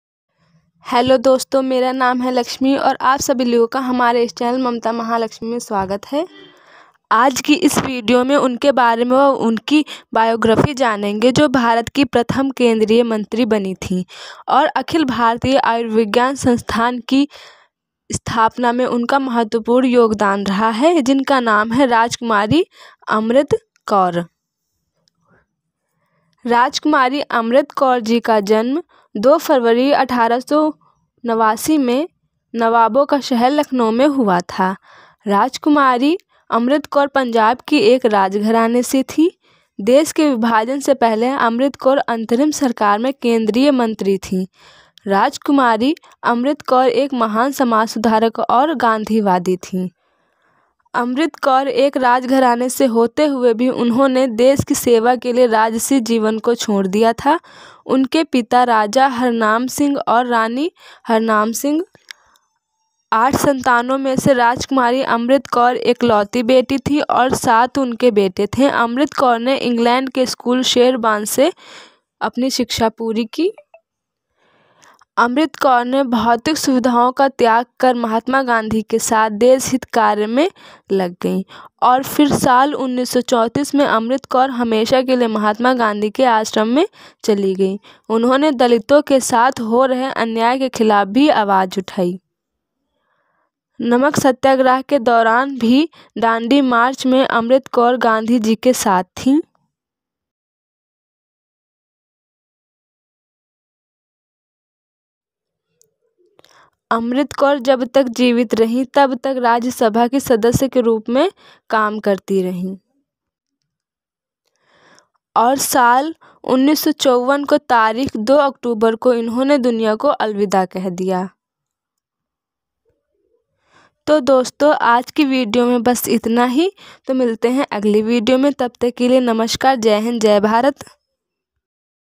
rajkumari amritkour biography|अमृतकौर और गांधी ने मिलकर आज़ादी की लड़ाई कैसे लड़ी।अमृतकौर#long वीडियो
अमृतकौर जी का जन्म 2 फरवरी 1889 में नवाबो के शहर लखनऊ में हुआ था राजकुमारी अमृतकौर पंजाब के एक राजघराने से थी देश के विभाजन से पहले अमृतकौर अंतरिम सरकार में केंद्रीय मंत्री थी अमृतकौर एक महान समाज सुधारक और गांधीवादी थी अमृतकौर ने देश की सेवा के लिए सामाजिक सुखों का त्याग कर दिया था।

long video#video#youtube video#essay#article